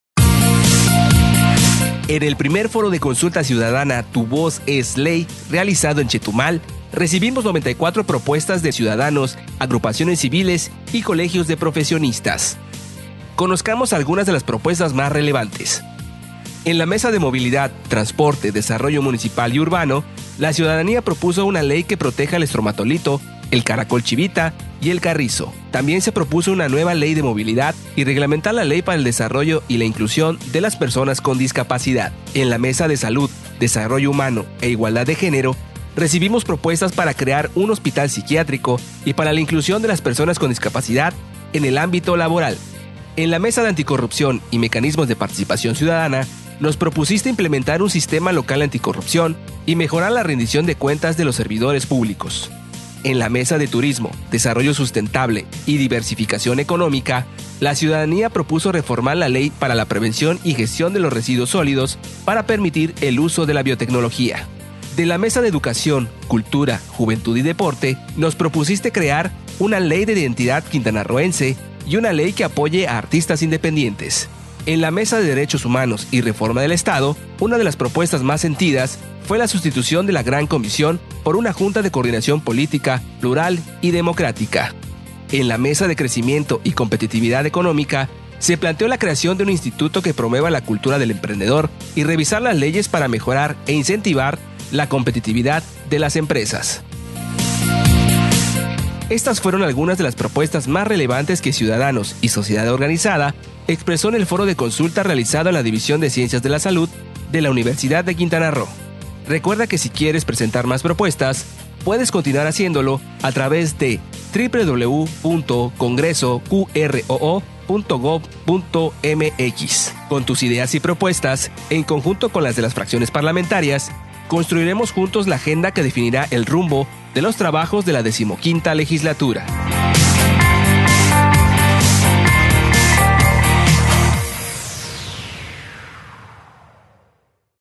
94 propuestas ciudadanas en Foro
Conozcamos las principales propuestas presentadas por la sociedad, en el primer Foro de Consulta Ciudadana TuVozEsLey realizado en Chetumal.